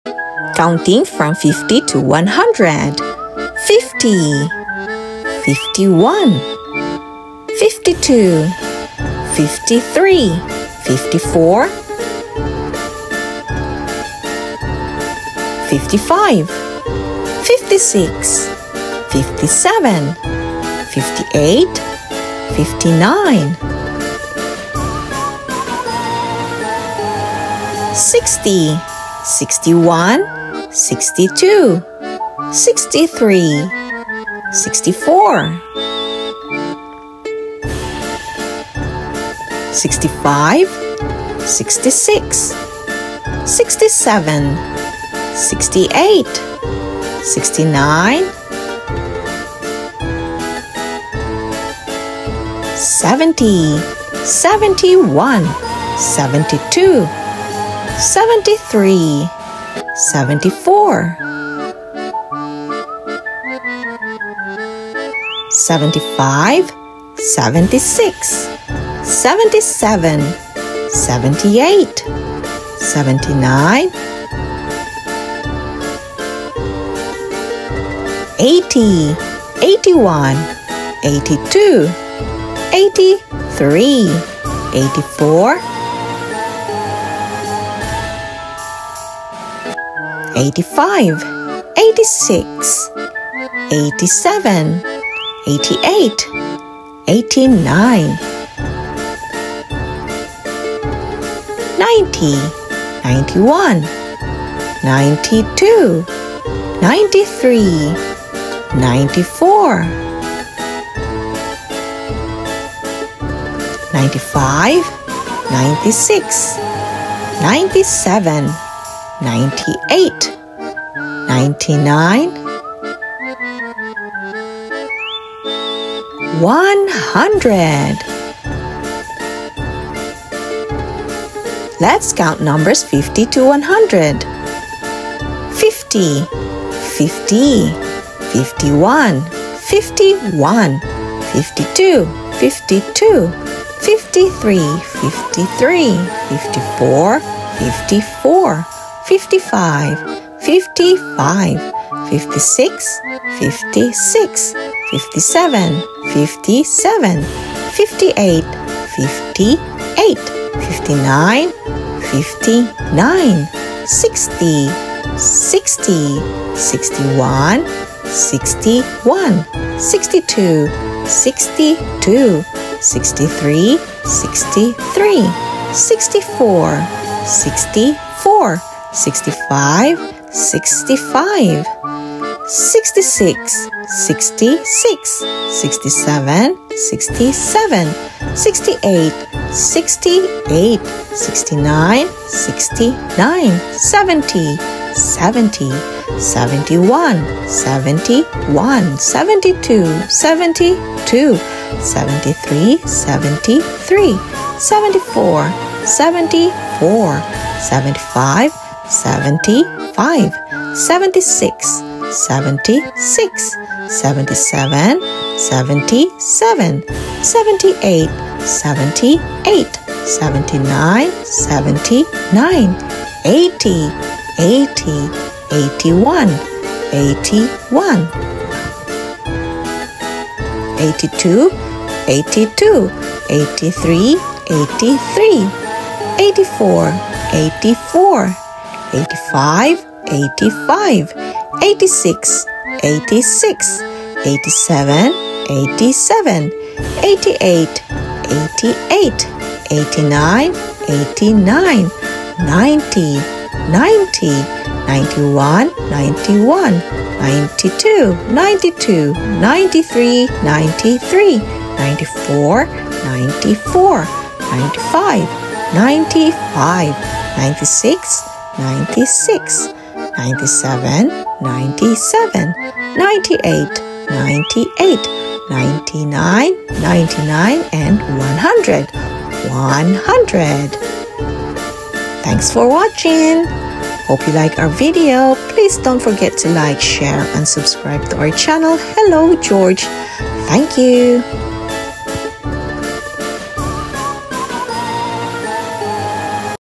NUMBERBLOCKS 50 TO 100 HALLOWEEN SPECIAL EDITION | LEARN TO COUNT | hello george
NUMBERBLOCKS 50 TO 100 HALLOWEEN SPECIAL EDITION | LEARN TO COUNT
Happy halloween to everyone and to all numberblocks friends and subscribers around the world. In today's video let's count numbers from 50 to 100 using numberblocks.